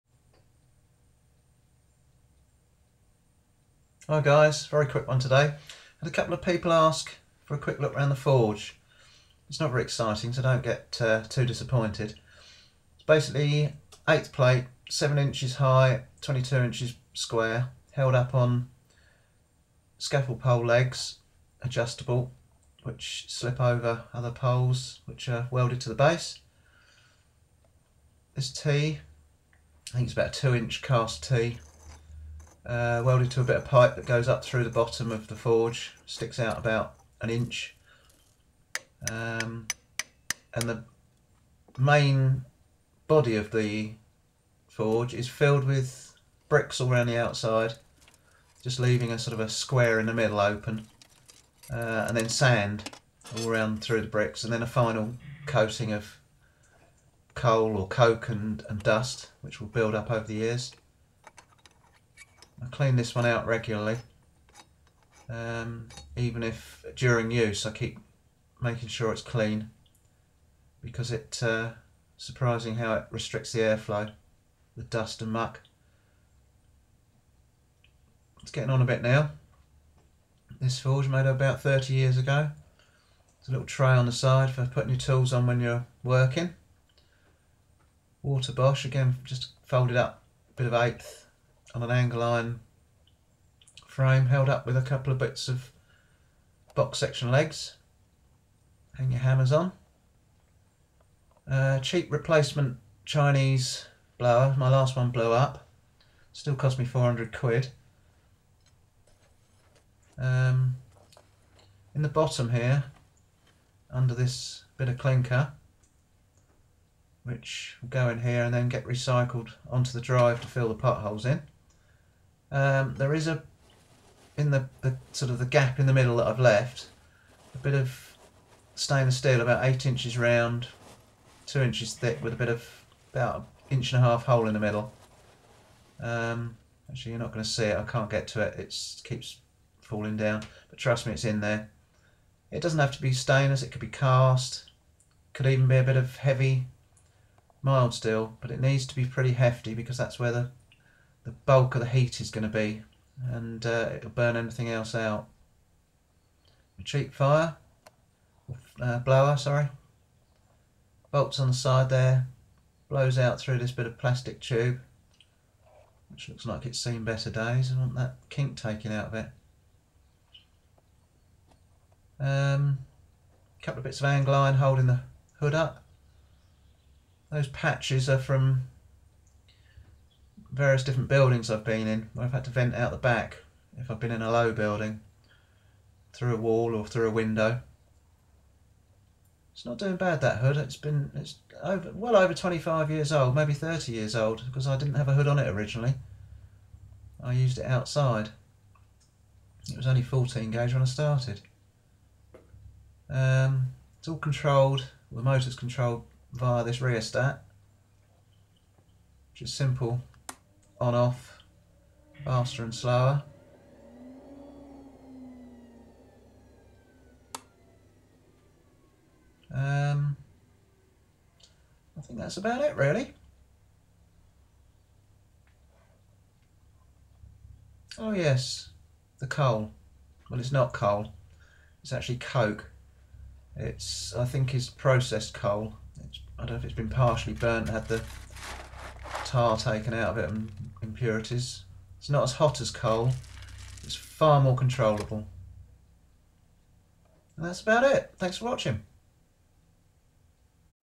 My Forge
A quick view around my 30 year old home built forge.